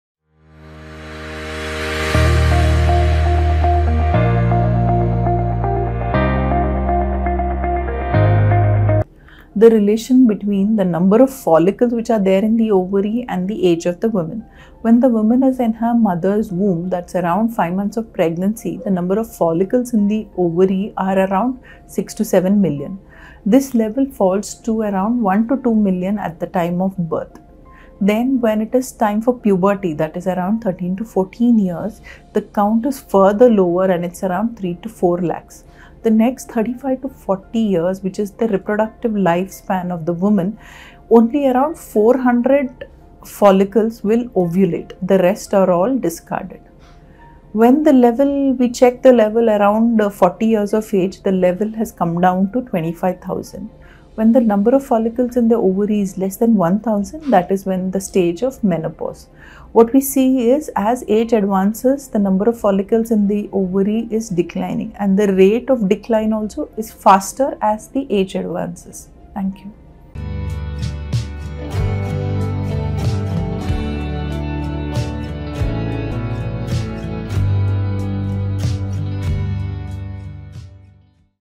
Egg count in females from birth to age of 40 | Dr. Soumya Shetty | Nova IVF Chembur
Watch this video by Dr. Soumya Shetty, IVF specialist at Nova IVF Chembur who explains Egg count in females from birth to age of 40